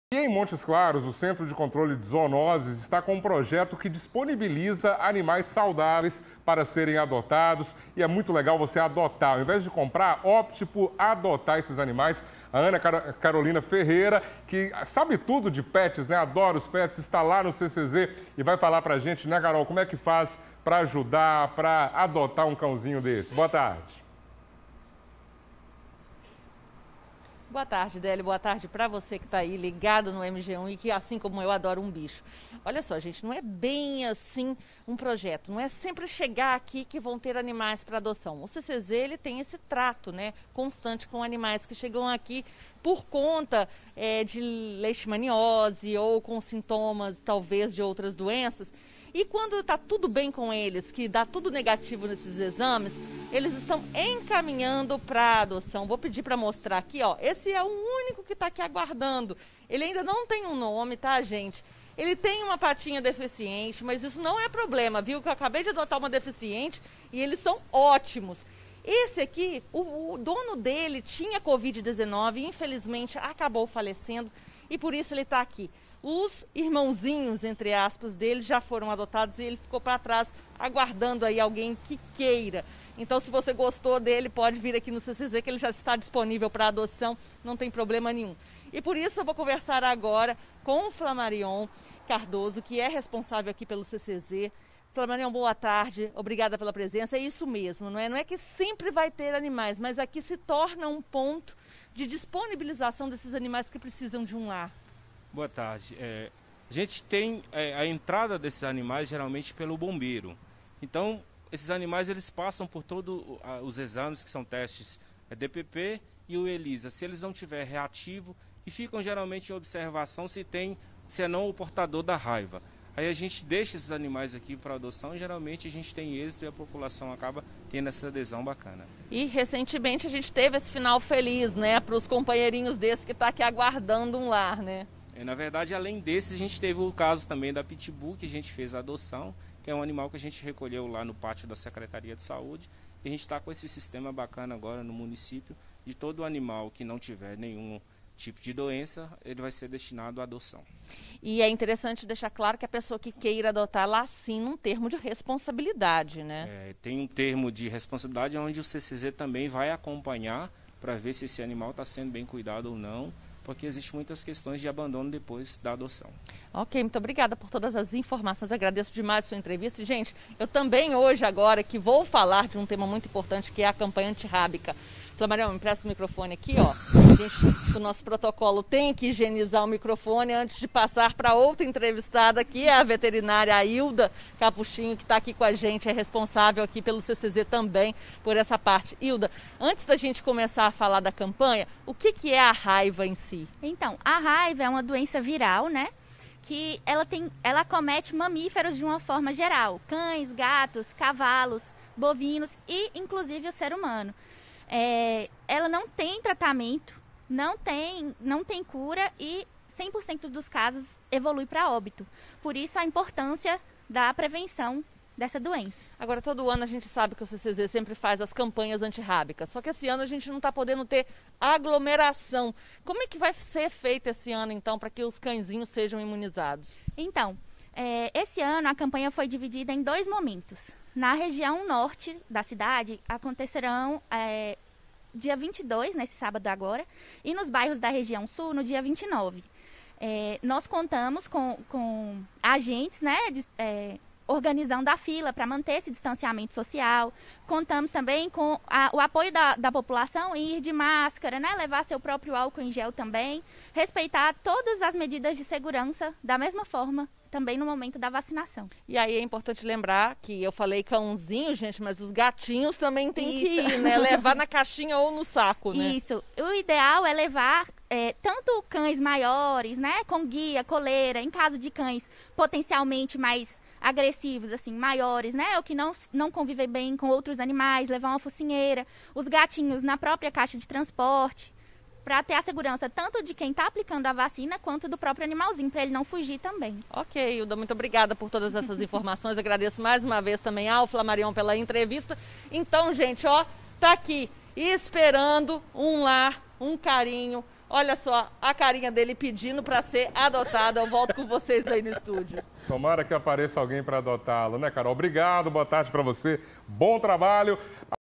Centro de Controle de Zoonoses inicia projeto de adoção de animais saudáveis
Para quem tiver interesse em adotar uma animal, é importante lembrar da responsabilidade de ter um cãozinho, não pode levar para casa pra devolver ou jogar na rua. Além disso, no próximo sábado (22), o CCZ inicia campanha de vacinação contra a raiva para os animais.

Reportagem: MG Inter TV 1ª Edição - Grande Minas